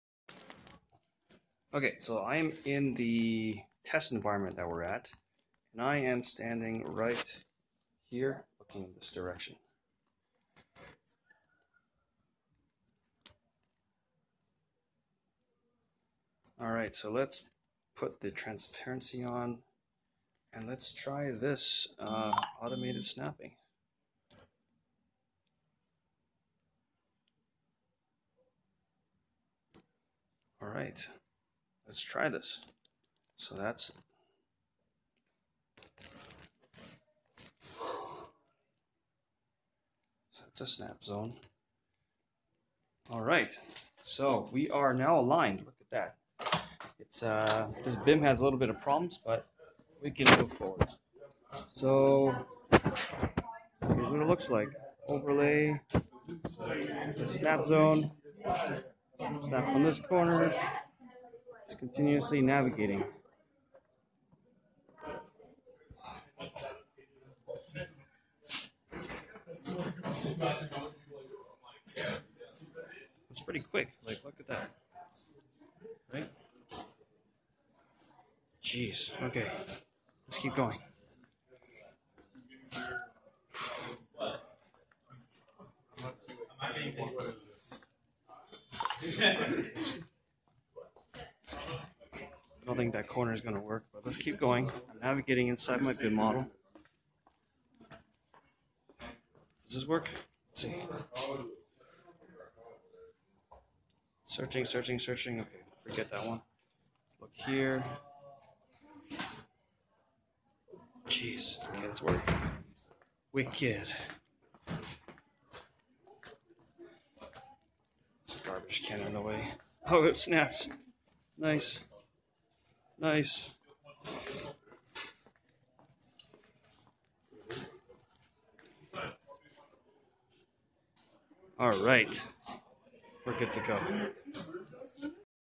Indoor Navigation Test using BIM and Google Tango | Augmented Reality | Mixed Reality | Shapetrace
This was a test that perform navigation drift-correction based on a BIM/3D CAD model. This was done with a Google Tango smartphone.

Prototype: March 23, 2017